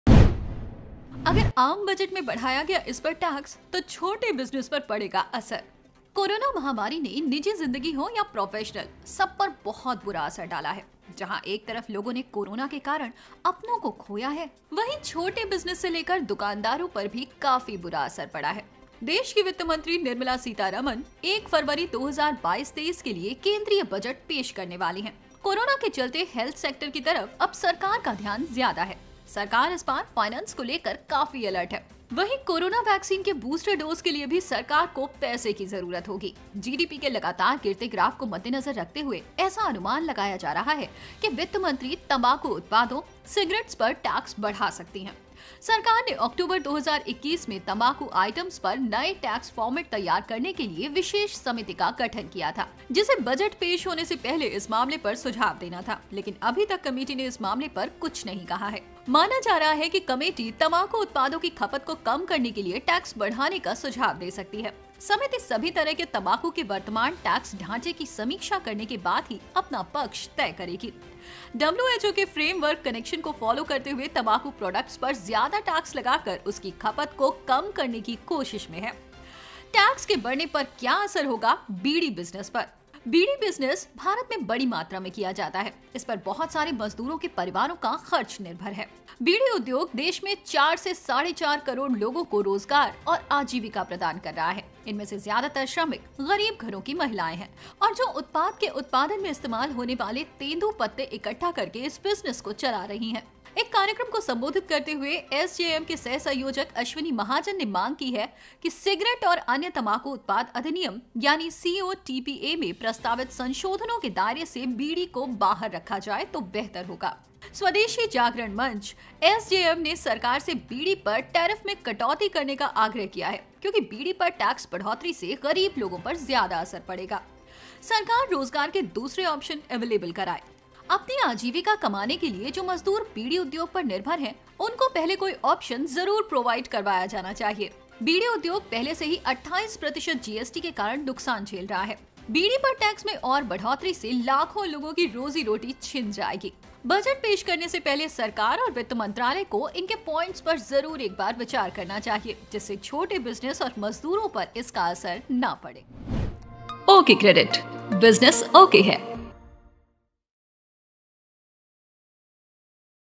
UNION BUDGET 2022 | जानें TAX बढ़ने से किसको हो सकता है नुकसान | OkCredit
1 Febraury को पेश होने वाला आम बजट | जानें टैक्स बढ़ने से किसको हो सकता है नुकसान | OkCredit

कोरोना महामारी ने निजी ज़िंदगी हो या प्रोफेशनल सब पर बहुत बुरा असर डाला है। जहां एक तरफ़ लोगों ने कोरोना के कारण अपनों को खोया है वहीं छोटे बिजनेस से लेकर दुकानदारों पर भी काफ़ी बुरा असर हुआ है।

देश की वित्त मंत्री निर्मला सीतारमण 1 फरवरी को 2022-23 के लिए केंद्रीय बजट पेश करने वाली हैं।

कोरोना के चलते हेल्थ सेक्टर की तरफ़ अब सरकार का ध्यान ज्यादा है। सरकार इस बार फाइनेंस को लेकर काफ़ी अलर्ट है वहीं कोरोना वैक्सीन के बूस्टर डोज के लिए भी सरकार के पैसे की जरुरत होगी। जीडीपी के लगातार गिरते ग्राफ़ को मद्देनज़र रखते हुए ऐसा अनुमान लगाया जा रहा है वित्त मंत्री तंबाकू उत्पादों, सिगरेट्स पर टैक्स बढ़ा सकती हैं।

सरकार ने अक्टूबर 2021 में तंबाकू आईट्म्स पर नए टैक्स फॉर्मेट तैयार करने के लिए विशेष समिति का गठन किया था जिसे बजट पेश होने से पहले इस मामले पर सुझाव देना था, लेकिन अभी तक कमिटी ने इस मामले पर कुछ नहीं कहा है।

माना जा रहा है कमिटी तंबाकू उत्पादों के खपत को कम करने के लिये टैक्स बढ़ाने का सुझाव दे सकती है। समिति सभी तरह के तंबाकू के वर्तमान टैक्स ढांचे की समीक्षा करने के बाद ही अपना पक्ष तय करेगी।

डब्ल्यूएचओ के फ्रेमवर्क कन्वेंशन को फॉलो करते हुये तंबाकू प्रोडक्टस पर ज्यादा टैक्स लगाकर उसके खपत को कम करने की कोशिश में है।

टैक्स के बढ़ने पर क्या असर होगा 'बीड़ी' बिज़नेस पर

बीड़ी बिजनेस भारत में बड़ी मात्रा में किया जाता है। इसपर बहुत सारे मज़दूरों के परिवार का खर्च निर्भर है। बीड़ी उद्योग देश में 4-4.5 करोड़ लोगों को रोजगार और आजीविका प्रदान कर रहा है। इनमें से ज्यादातर श्रमिक गरीब घरों की महिलाएं हैं और जो उत्पाद के उत्पादन में इस्तेमाल होने वाले 'तेंदू' पत्ते इकट्ठा करके इस बिज़नेस को चला रही हैं।

एक कार्यक्रम को संबोधित करते हुए एसजेएम के सह-संयोजक अश्विनी महाजन ने मांग की है सिगरेट और अन्य तंबाकू उत्पाद अधिनियम (सीओटीपीए) में प्रस्तावित संशोधनों के दायरे से 'बीड़ी' को बाहर ही रखा जाए तो बेहतर होगा।

स्वदेशी जागरण मंच (एसजेएम) ने सरकार से 'बीड़ी' पर टैरिफ में कटौती करने का आग्रह किया है। क्योंकि बीड़ी पर टैक्स बढ़ोतरी से गरीब लोगों पर ज़्यादा असर पड़ेगा।

सरकार रोजगार के दूसरे ऑप्शन अवेलेबल करवाएं

अपनी आजीविका कमाने के लिए जो मज़दूर बीड़ी उद्योग पर निर्भर हैं उनको पहले कोई ऑप्शन ज़रूर प्रोवाइड करवाया जाना चाहिए।

बीड़ी उद्योग पहले से ही 28 प्रतिशत जीएसटी के कारण नुकसान झेल रहा है। बीड़ी पर टैक्स में और बढ़ोतरी से लाखों लोगों की रोजी-रोटी छिन जाएगी।

बजट पेश करने से पहले सरकार तथा वित्त मंत्रालय को इन की पॉइंट्स पर ज़रूर एक बार विचार करना चाहिए। जिससे छोटे बिज़नेस और मजदूरों पर इसका असर ना पड़े।